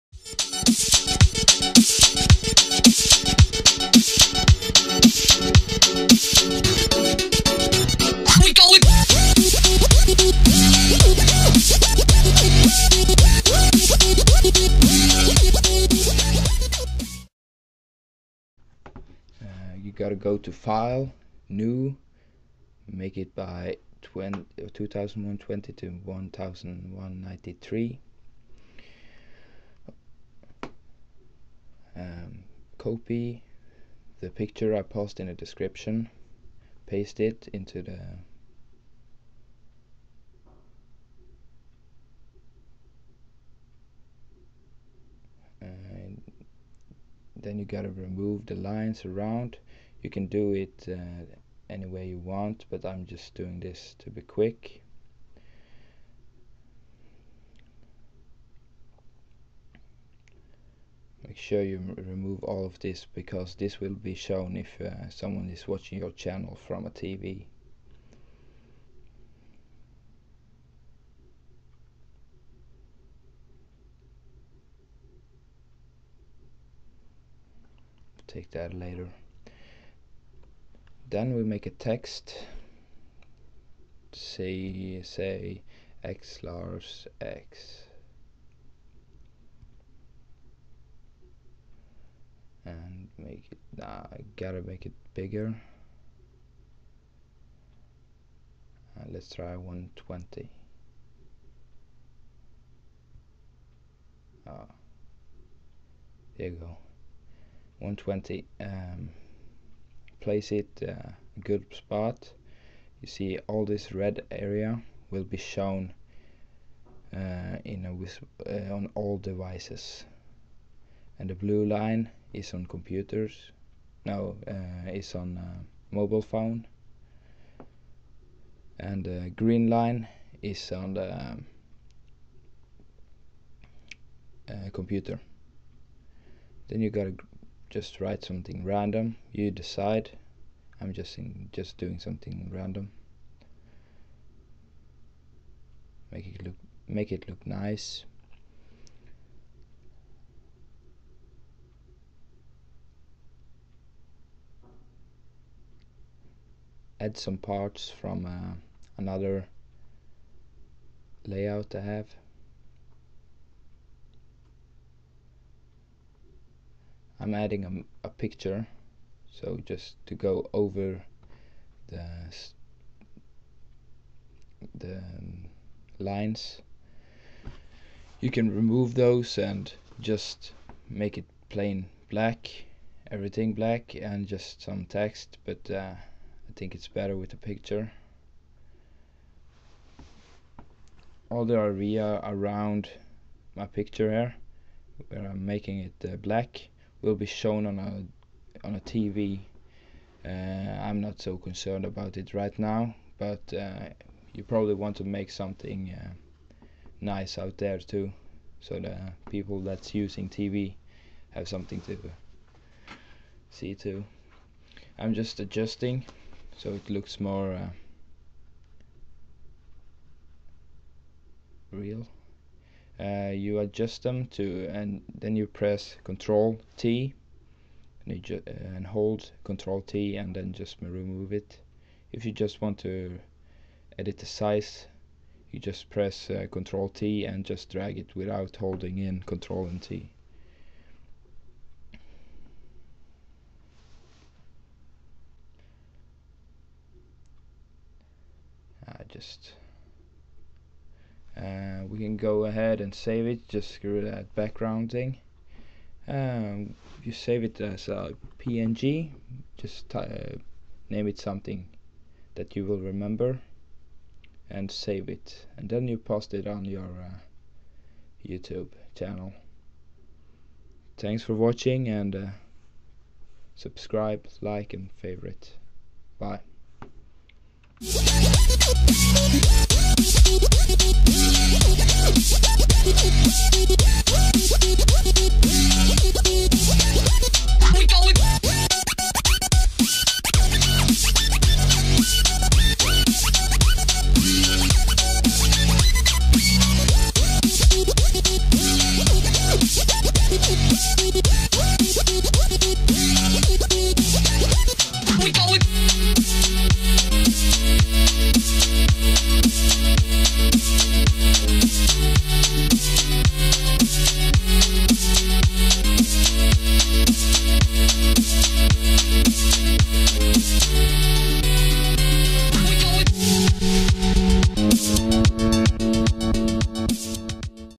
HOW EASY MAKE A CHANNEL ART 2017
I'm using Photoshop CS6, if u don't have it you can use GIMP. That's a free software and it's similar to Photoshop :)
Link to template picture "COPY THAT AND PASTE INTO YOUR PHOTOSHOP!"